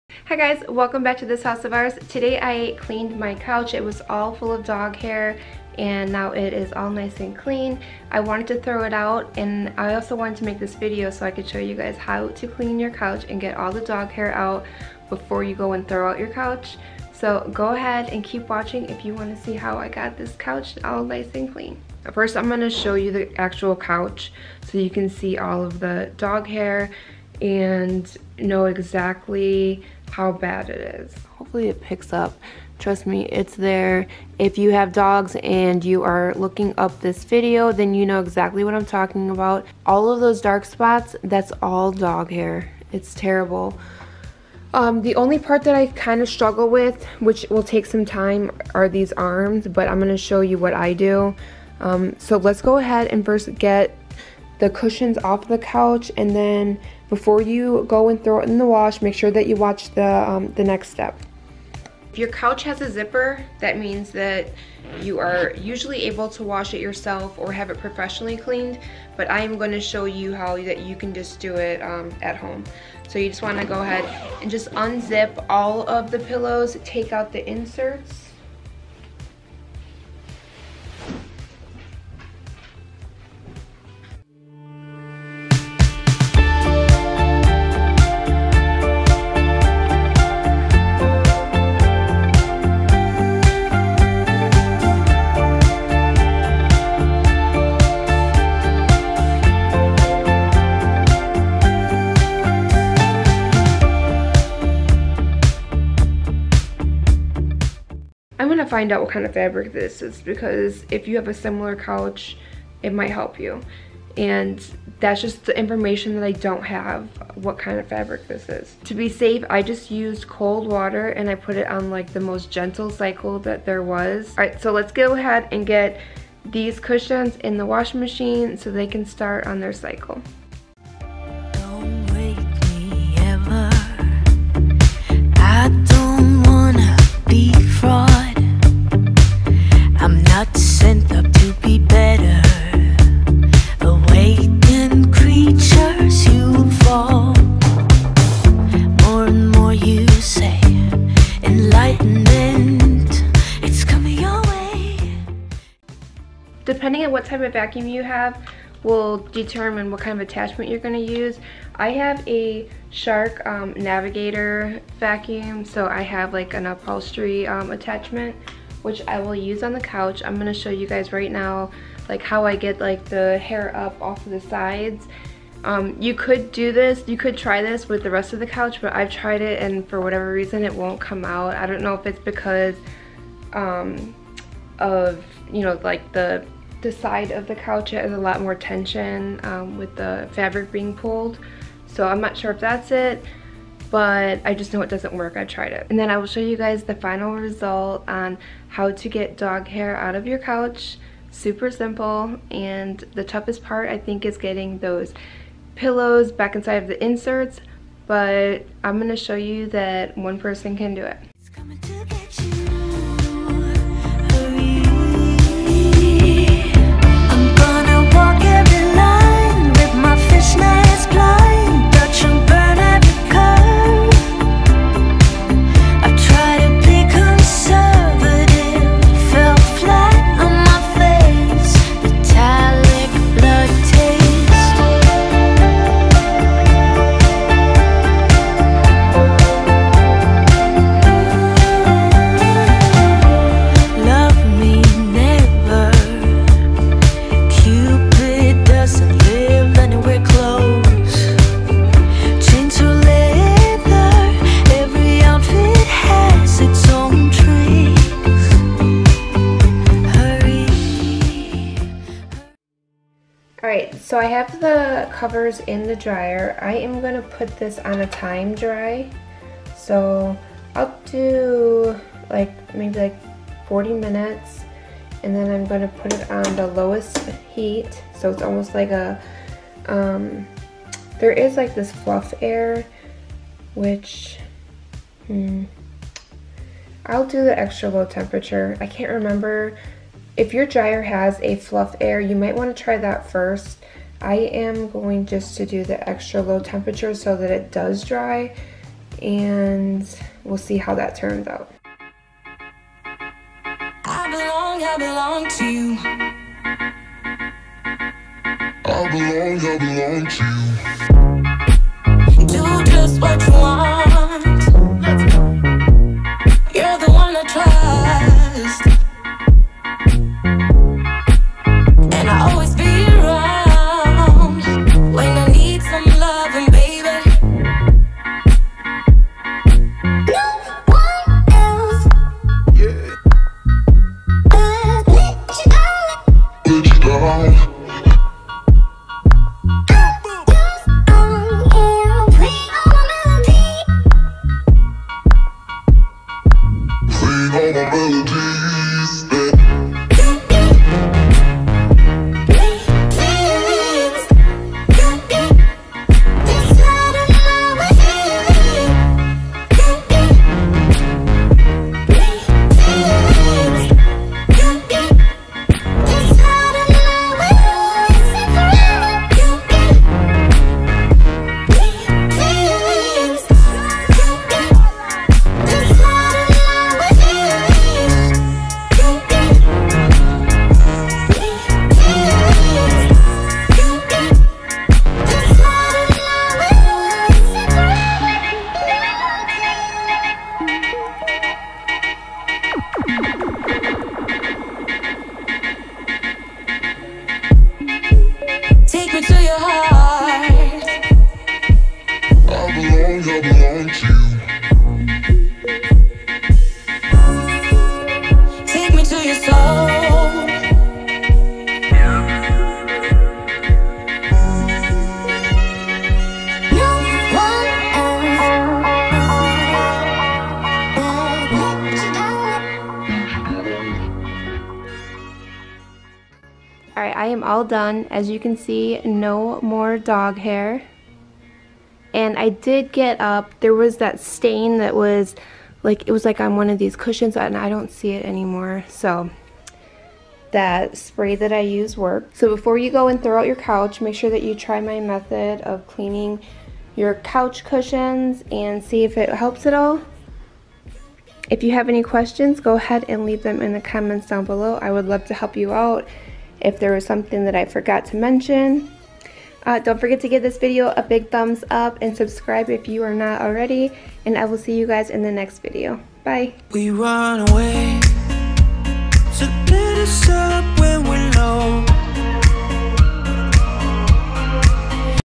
HOW TO CLEAN PET FUR OFF OF COUCH 2019 | EASY DIY COUCH CLEANING | This House of Ours
Watch before you throw out your couch :) I know it seems impossible to get your pets fur and hair out of the couch, but I promise its worth a try. i will show you how to clean you couch, so it looks good as new.